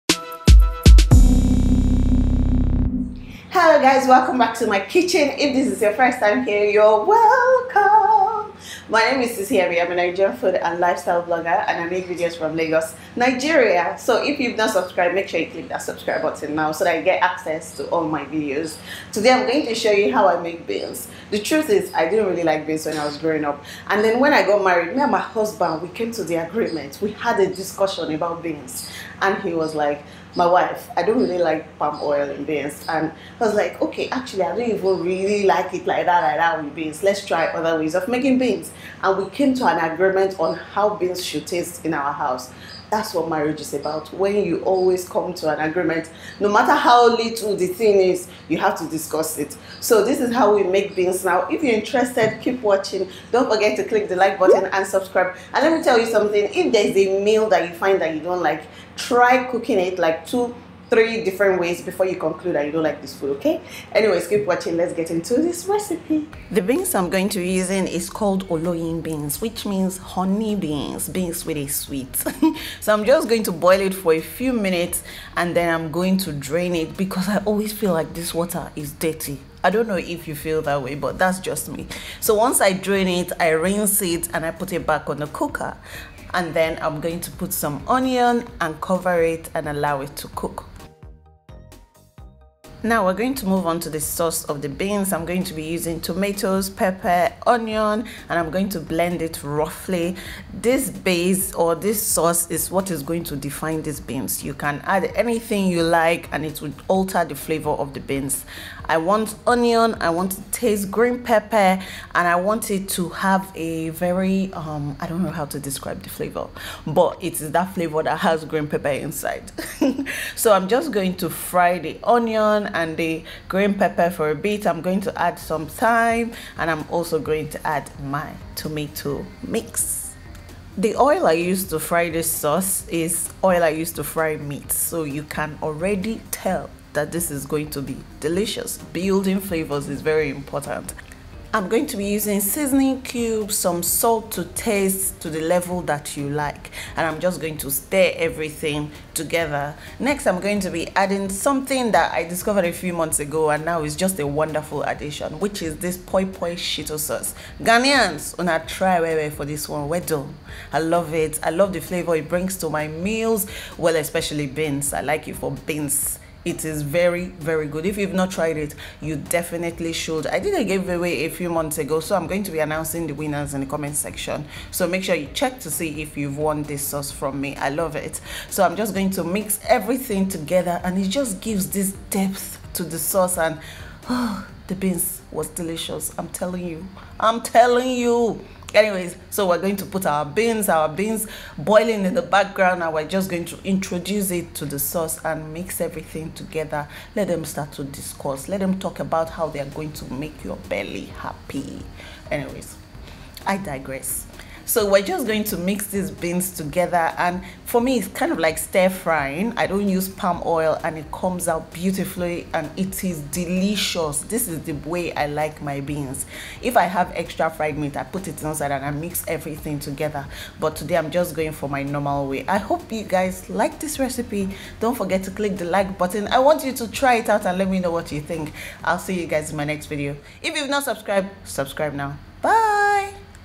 NIGERIAN BEANS | NO PALM OIL
I started cooking beans this way because Bobo was not "feeling" palm oil beans plus this version in my opinion is moreeeee delicious! The winners for the PoiPoi Sauces are:
Debbie Briggs
Yemisi Daramola
Hannah Onoguwe
Valada Fakuade
Ijeoma Ubaneche

Location: Lagos, Nigeria (West Africa)
I'm a Nigerian Food and Lifestyle Blogger documenting bits of every other day in my life with my son, Tito and husband Bobo.